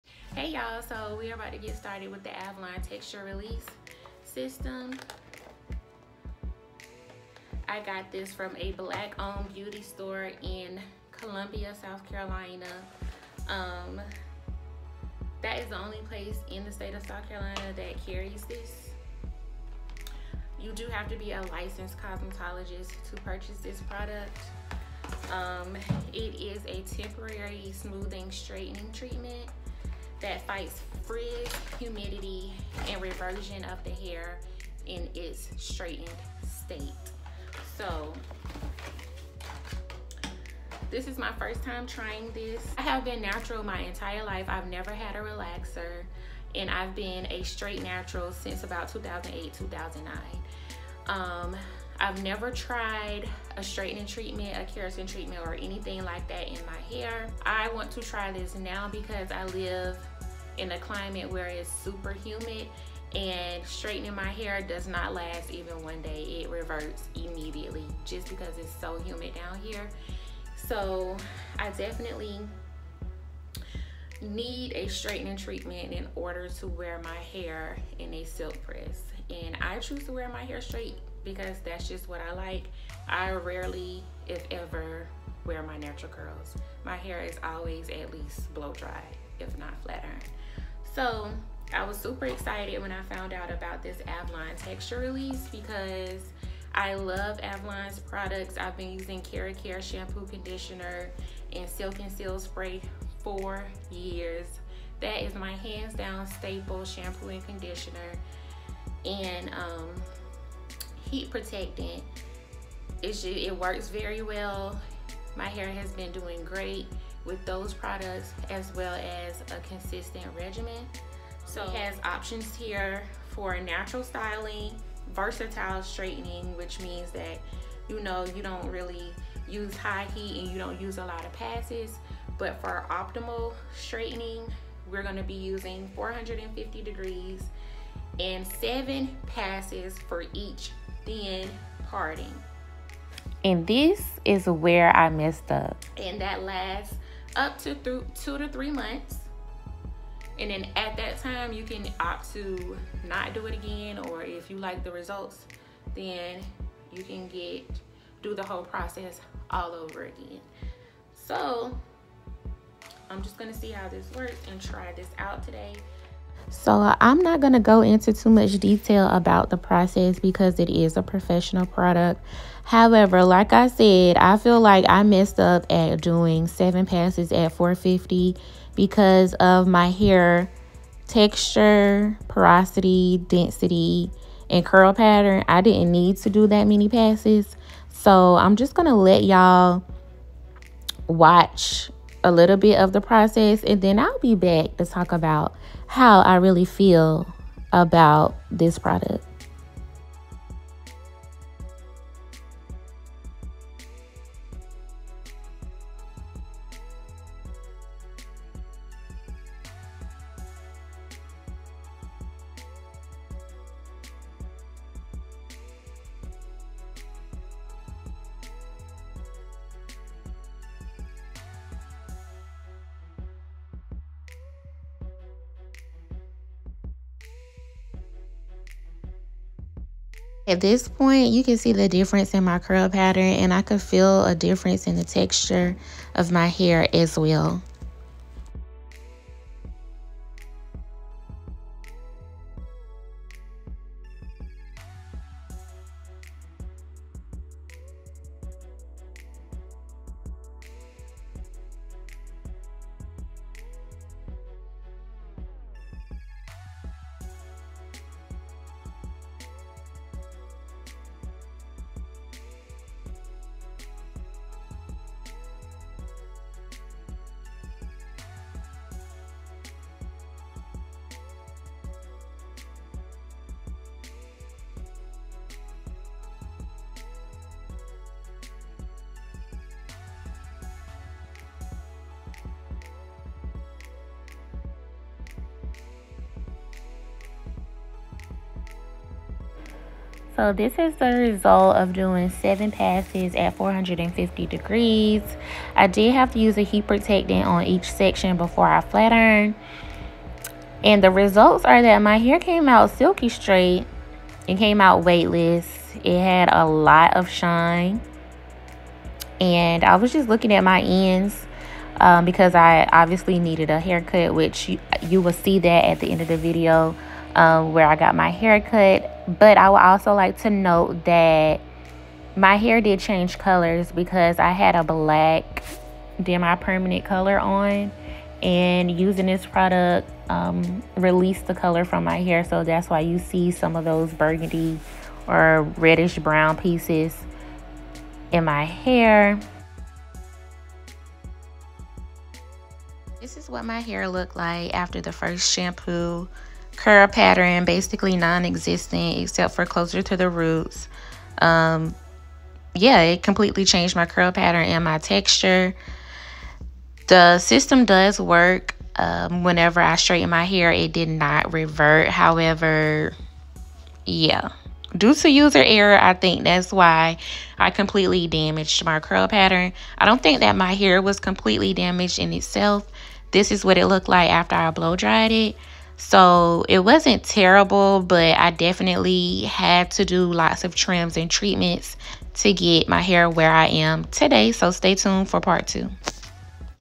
Avlon Texture Release | It permanently changed my curl pattern Part 1 | SimplyShakeila
Hey y’all this is Part 1 of 2 of my review of the Avlon Texture Release.